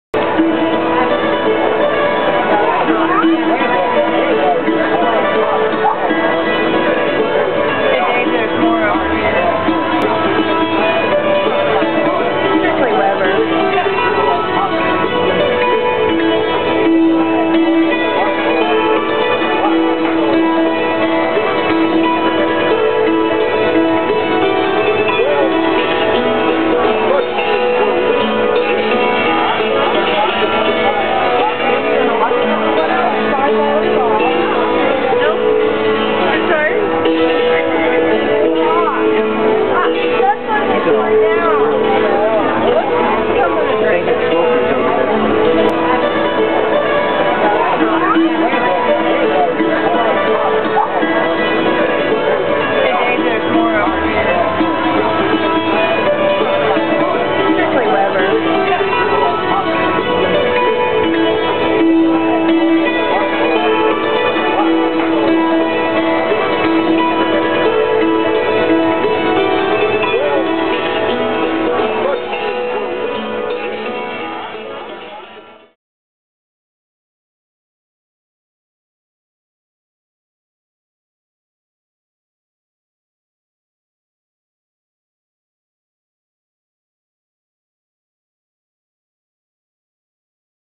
RAY BROOKS AT THE FARMERS' MARKET
Hammered dulcimer player Ray Brooks (one-time Eastern Regional Champion) plays at the Worthington, Ohio Farmers' Market - May, 2007